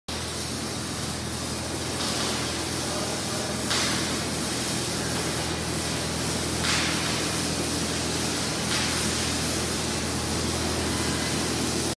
Single girder overhead crane 5 ton in Puerto Rico
On June 11, 2019, Puerto Rico Construction and Prefabrication Company gave Clescrane the contract of 2 sets of single girder bridge crane (CHS5T-15.945m H=3.688m A5/M5), rail beams, and installation guide services for 10 days.